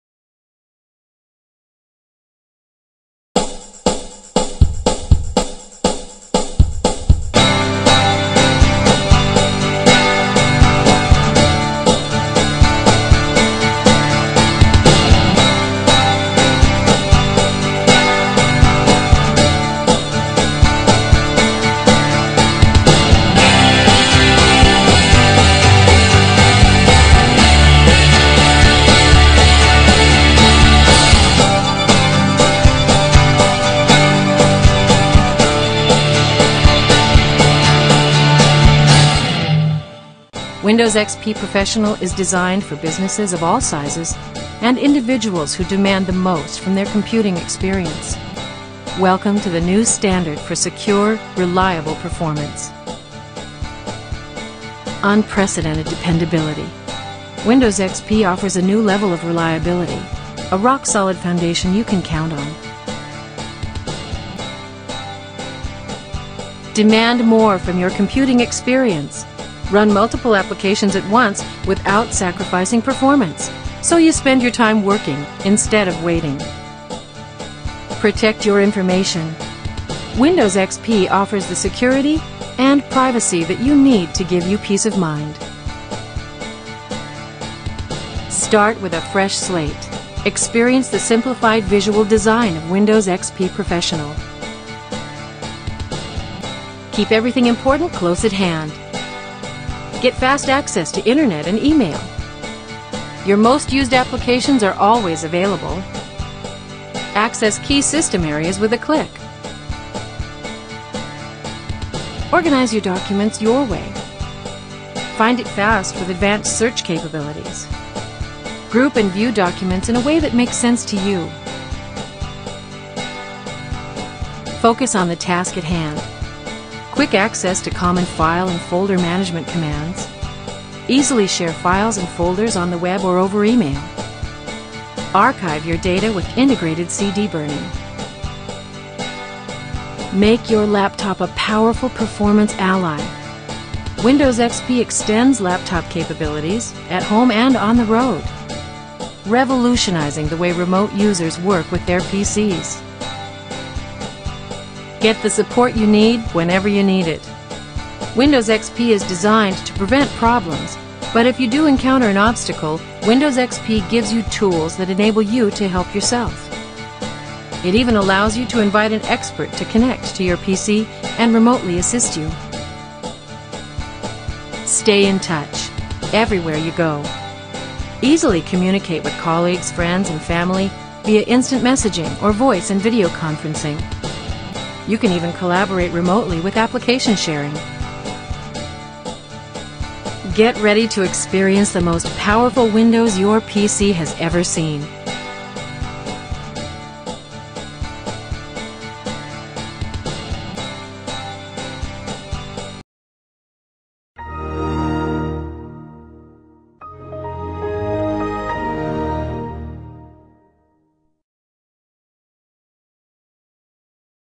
Windows XP Professional Overview - Flash Tour
Here's an obscure flash overview tour for Windows XP Professional, in English.